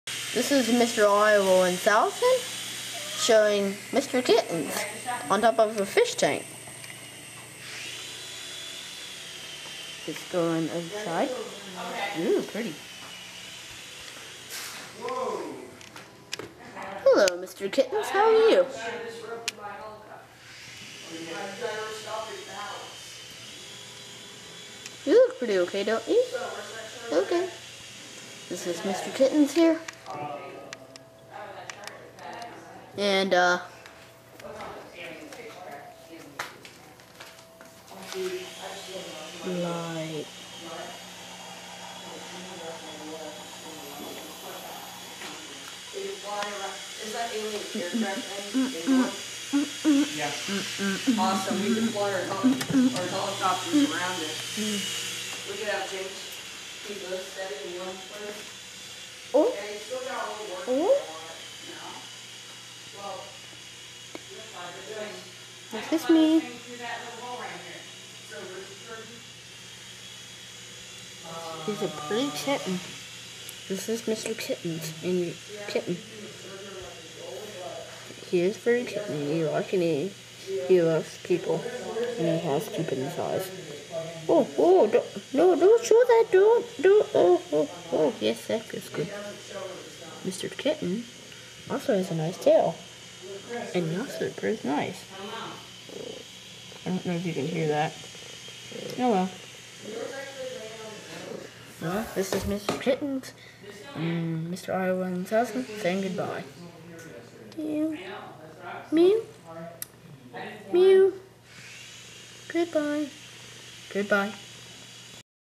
Mr. kitttens
This is my friend's cat mr. kittens. Is't he nice?